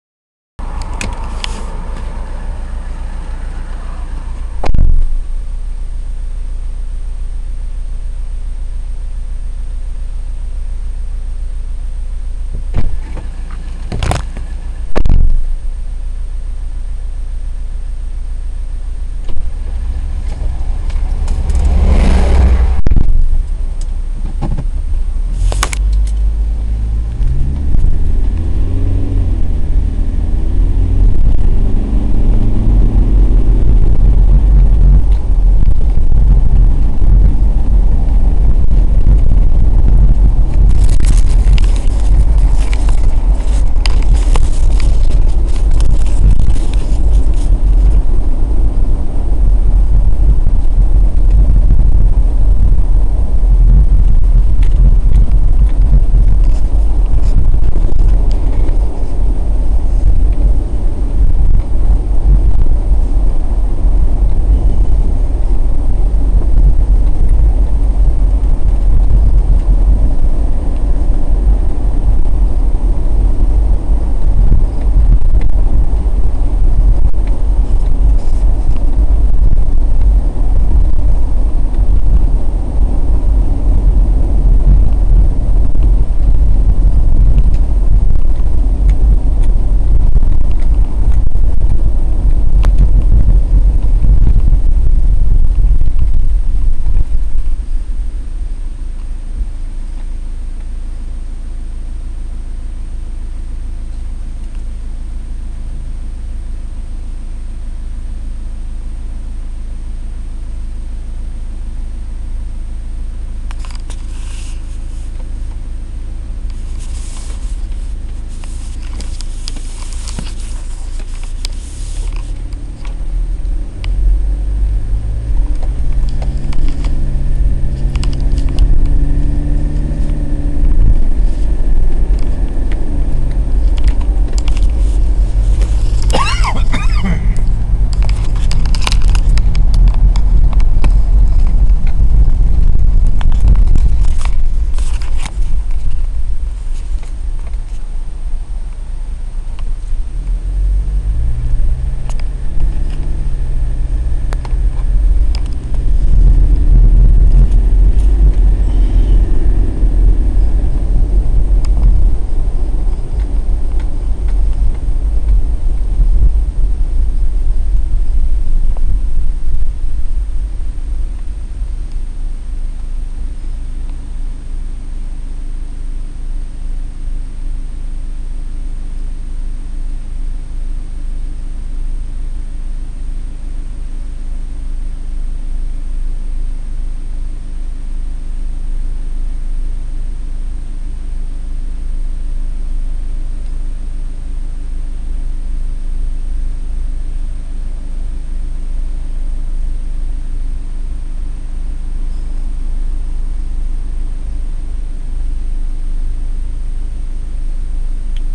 H720-III-21EHR43.AVI
Mini DVR H720III with bullet camera 21EH-R43 connected. Better car option would be 6mm or 8mm [to remove sky/car front]. Supplied by RF Concepts Ltd.